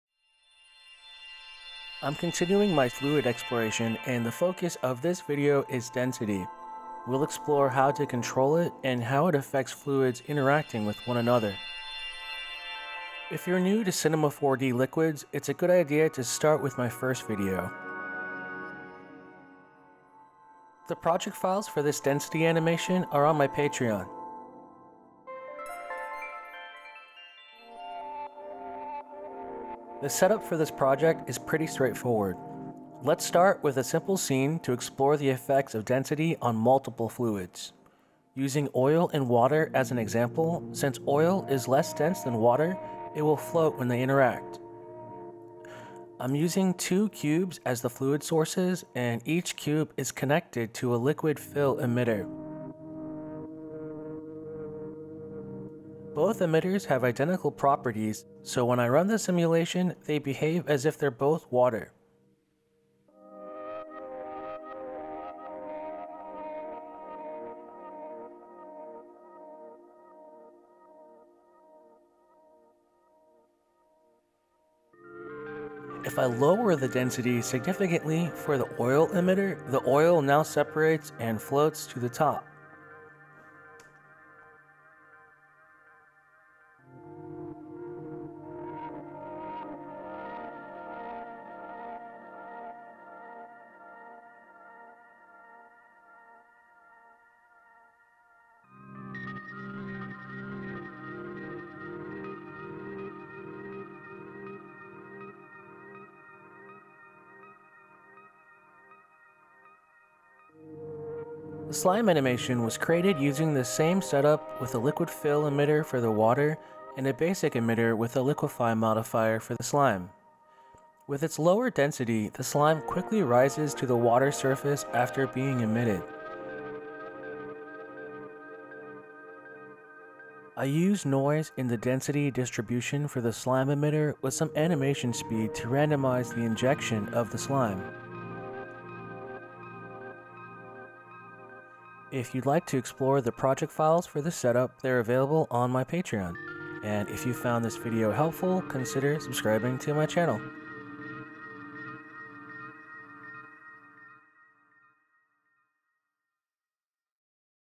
Exploring Liquid Density in Cinema 4D 2025
The release of Cinema 4D 2025.3 introduced a new fluid simulation system. In this video I explore how to work with density and how it affects fluids interacting with one another.